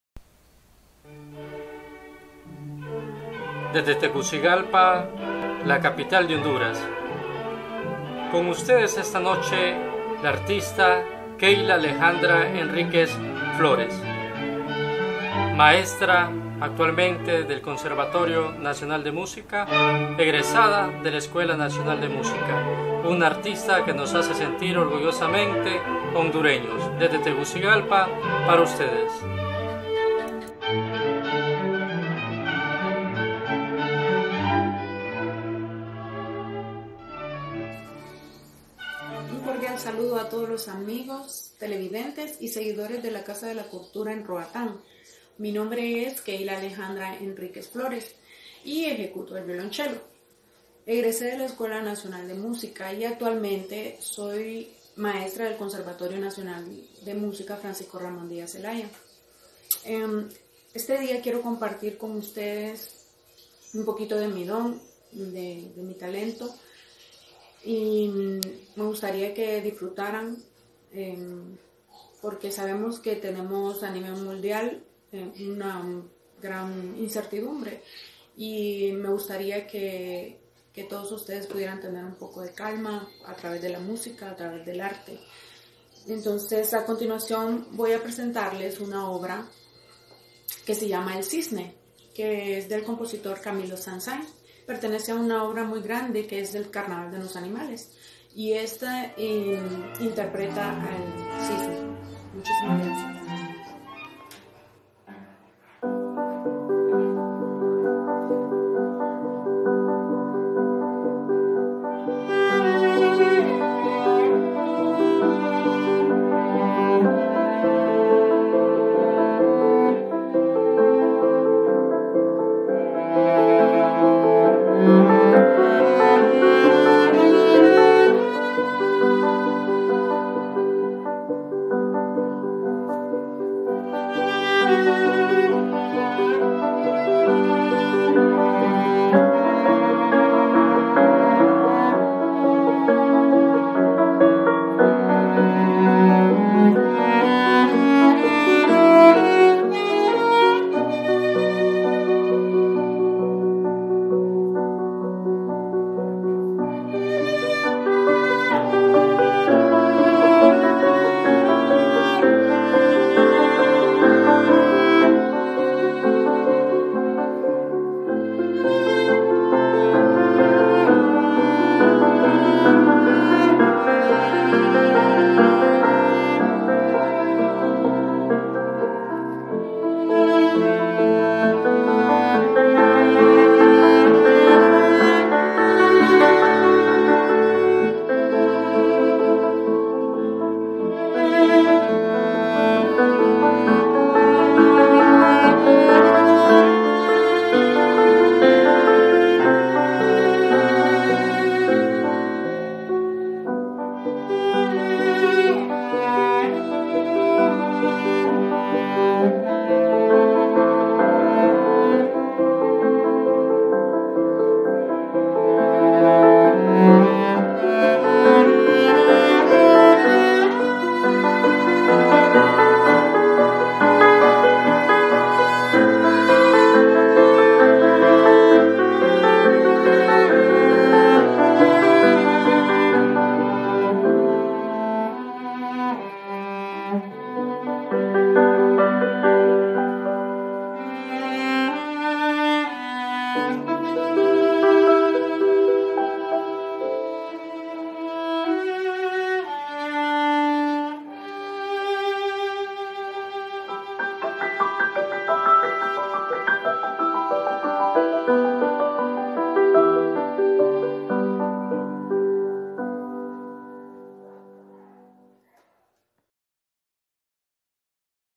ISLAS DE LA BAHIA CULTURA Y PATRIMONIO CON JHON GIRON MANZANARES
KEILA ENRIQUEZ MAESTRA DE MUSICA EN LA ESPERIMENTAL DE NIÑOS